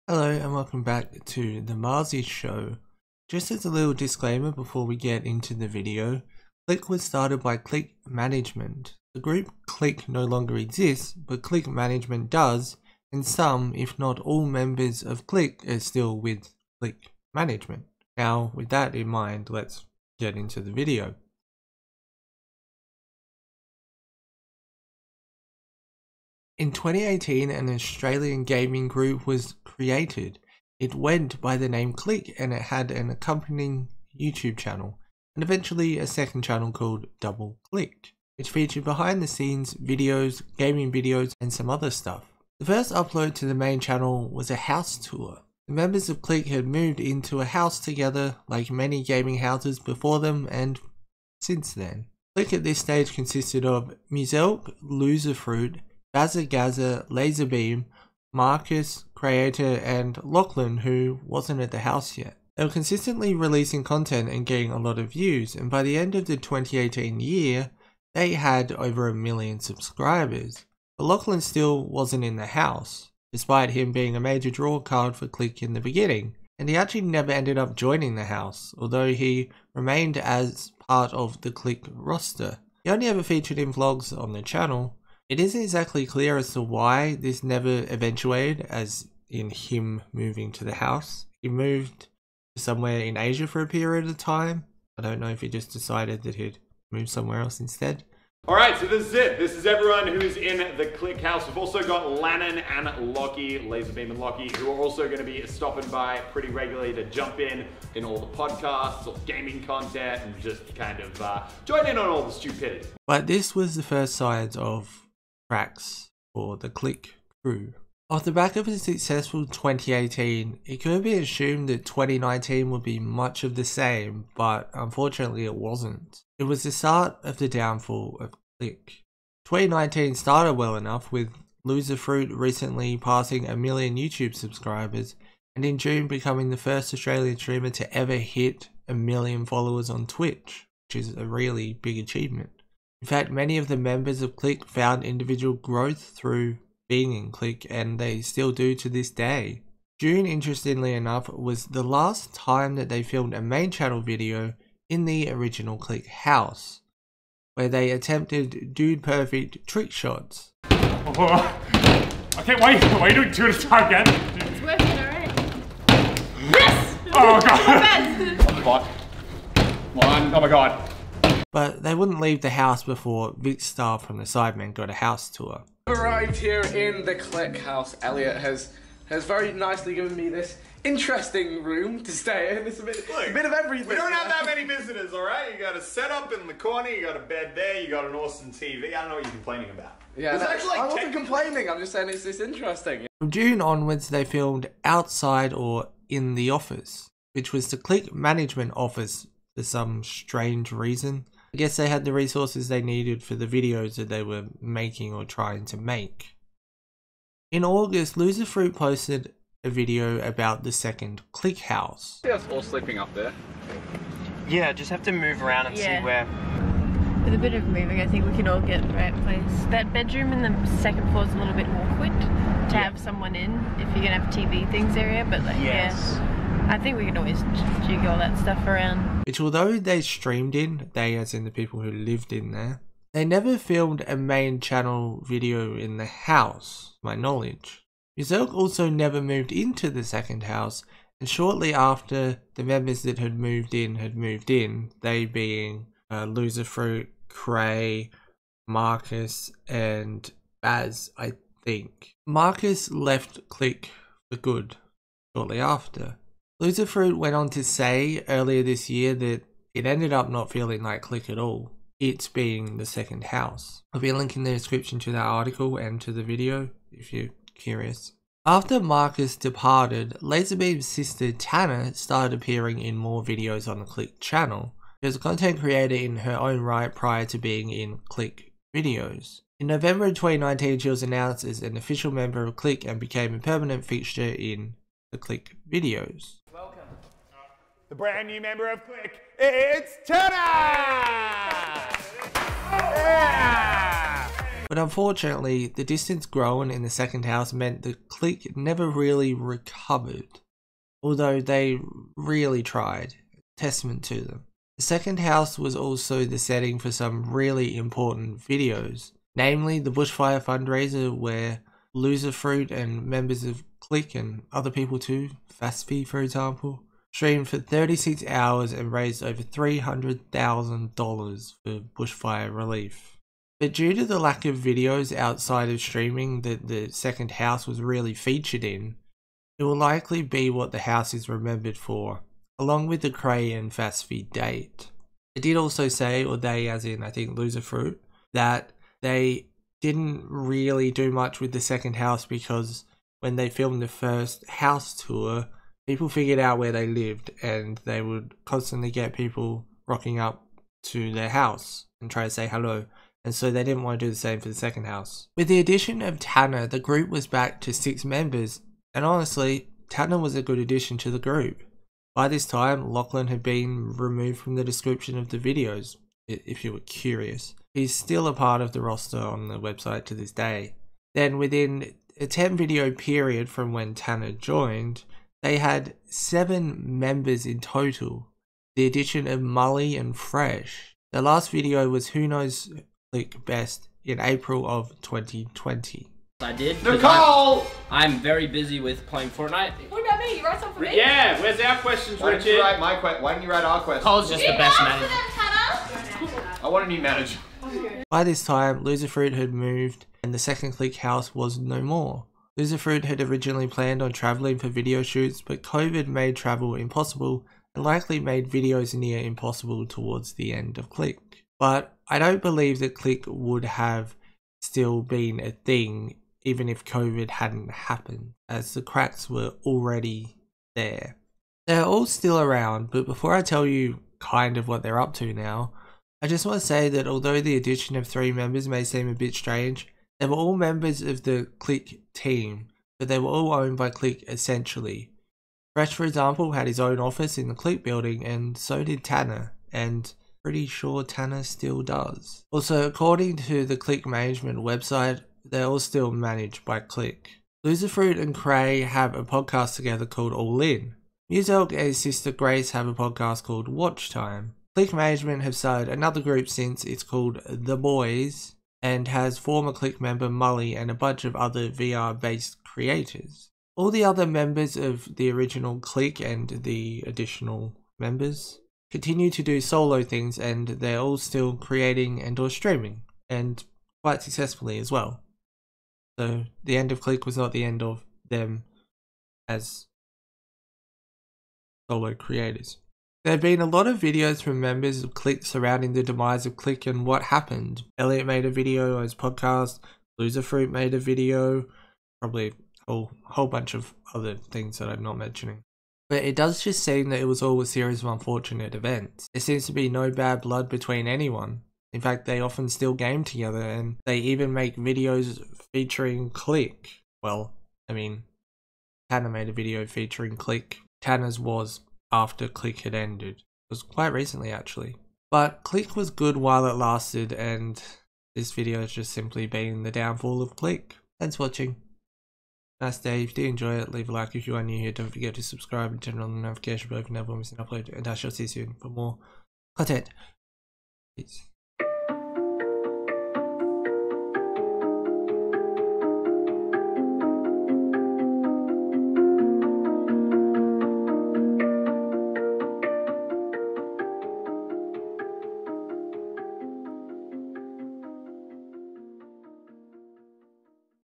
Click Crew: The Downfall Of
The Click Crew, also known simply as Click, were an Australian based gaming group formed in 2018. The members were Loserfruit, Muselk, BazzaGazza, Crayator, Lazarbeam and Prestige/Marcus. This video looks at how click started, how click ended, how they tried to save click and everything in between.